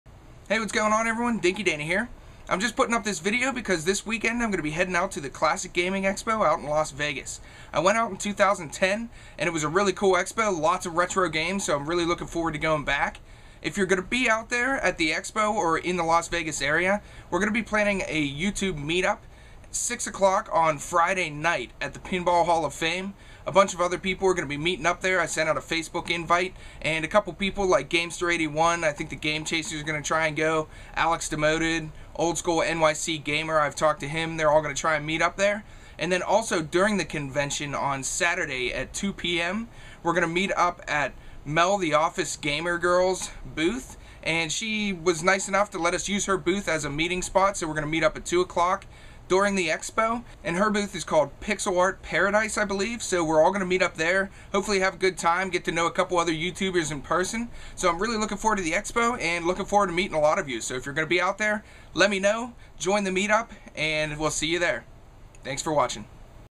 Classic Gaming Expo Meetup - Las Vegas (August 10-12)
I'm going to be out in Las Vegas this weekend (August 10-12) for the Classic Gaming Expo.  We're planning a YouTube meetup Friday at 6pm at the entrance of the Pinball Hall of Fame so if you're in the area, head on over.

There will also be a meetup inside the Classic Gaming Expo Saturday at 2pm at Mel the OfficeGamerGirls booth called Pixel Art Paradise.  The conventions gonna be a blast and I'm looking forward to meeting some fellow YouTubers.  See you there!